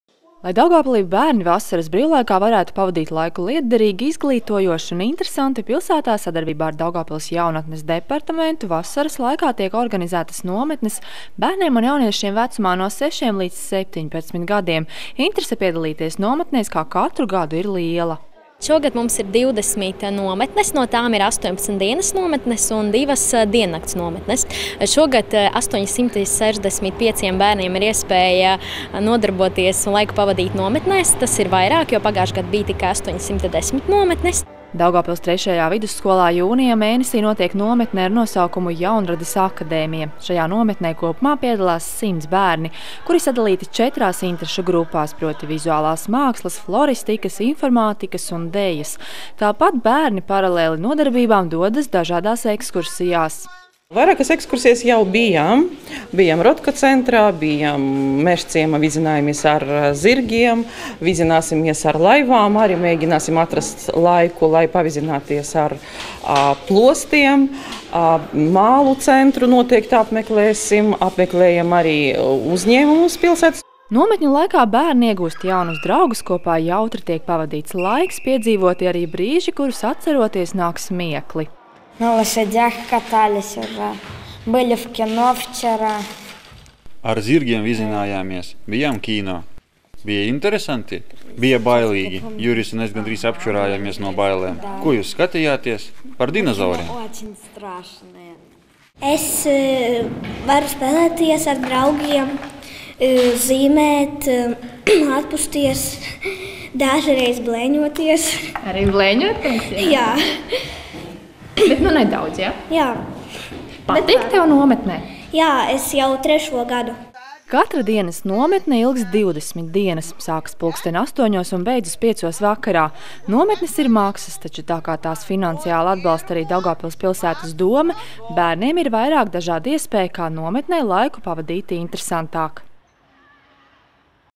Vasara, laiks bērnu nometnēm
Lai Daugavpilī bērni vasaras brīvlaikā varētu pavadīt laiku lietderīgi, izglītojoši un interesanti, pilsētā sadarbībā ar Daugavpils Jaunatnes departamentu vasaras laikā tiek organizētas nometnes bērniem un jauniešiem vecumā no sešiem līdz septiņpadsmit gadiem. Interese piedalīties nometnēs, kā katru gadu, ir liela.